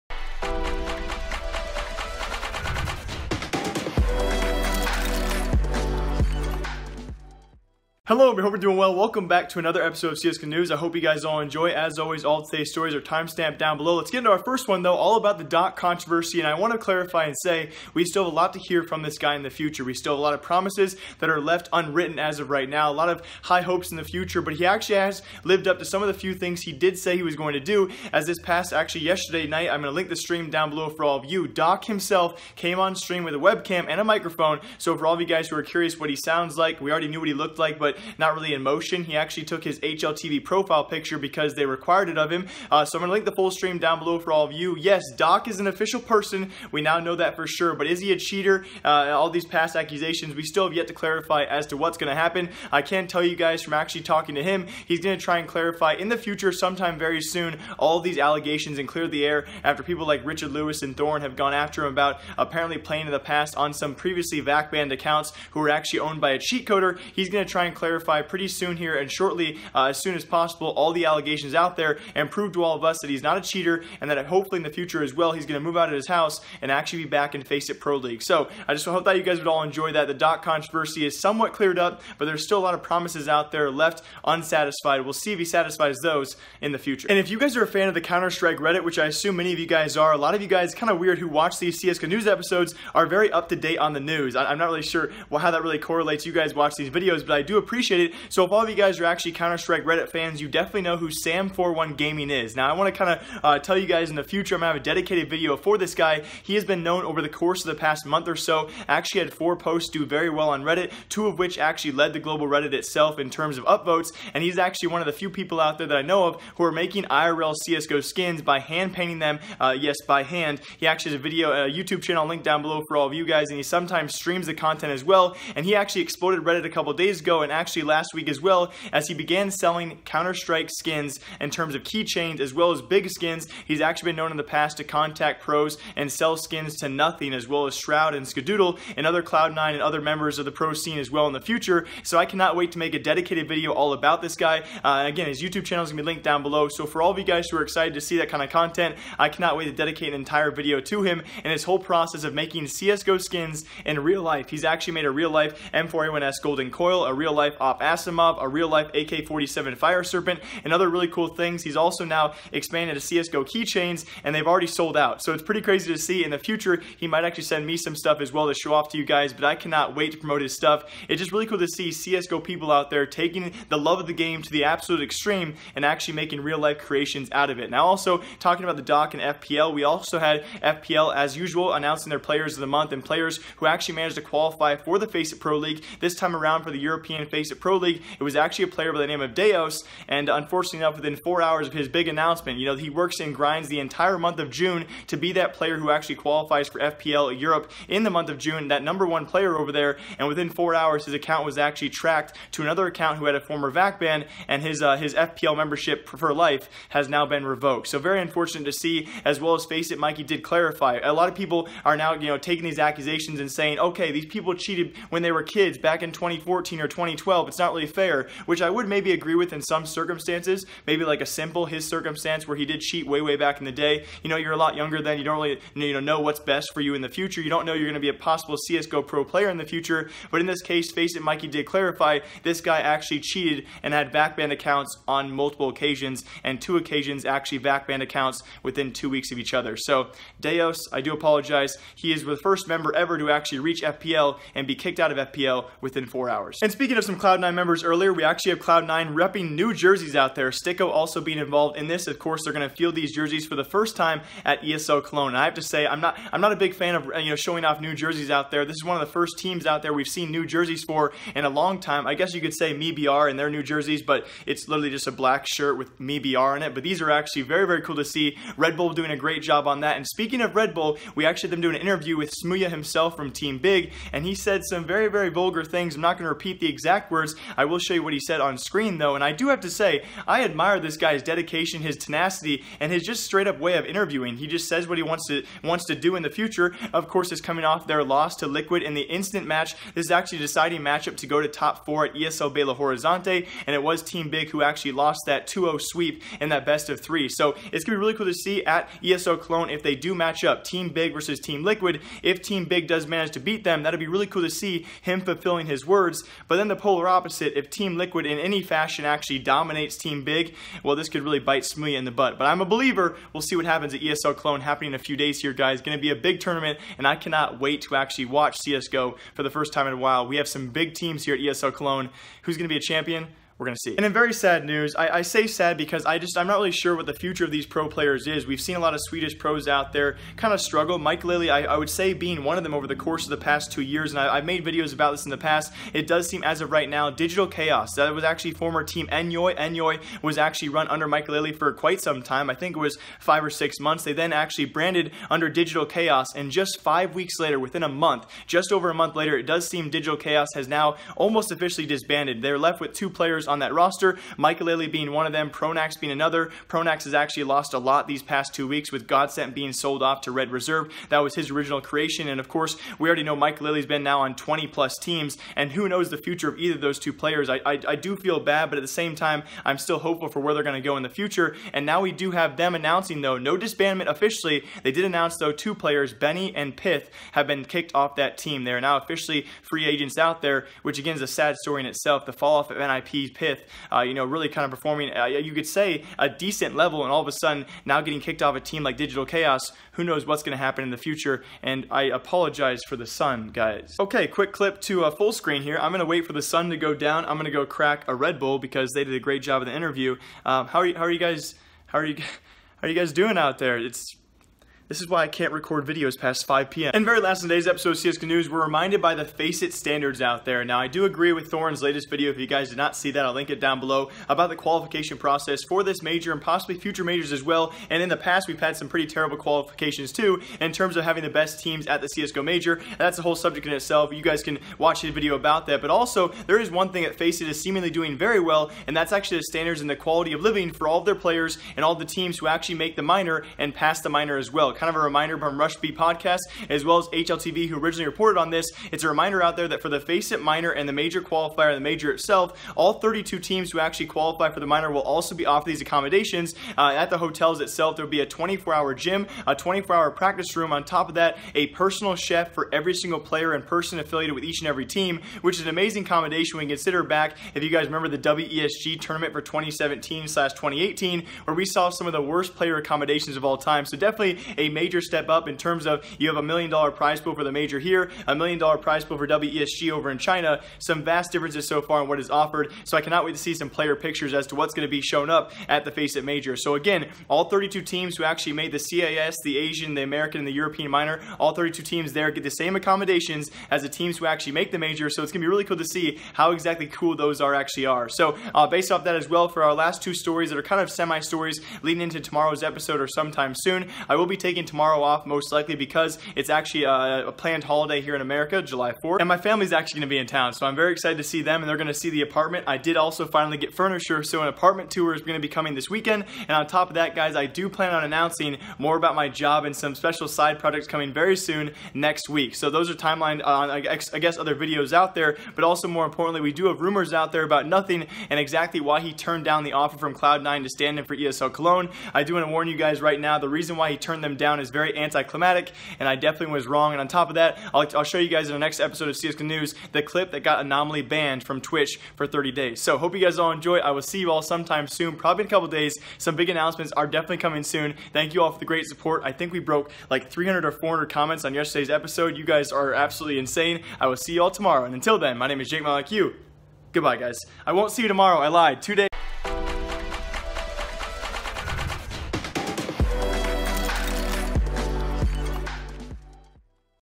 D0cC Finally Speaks and Streams! Smooya Talks Trash About Liquid, Real Life CSGO Skins and More!
Follow me on Instagram D0cC finally decides to prove his promises, C9 shows off new jerseys, Smooya talks the talk but can he walk the walk, this is how CSGO skins are made in real life and more!
00:15 - D0cC Speaks and Streams!
1:26 - CSGO Skins Made in Real Life!
2:58 - Newest FPL Member Banned Within 4 Hours...
4:06 - C9's Newest Team Jerseys Are Sick
4:29 - Smooya's Interview About Team Liquid
5:23 - Pyth and Benny Kicked from Digital Chaos... Disband Soon?
6:47 - Faceit Major to Have Awesome Accommodations for Pros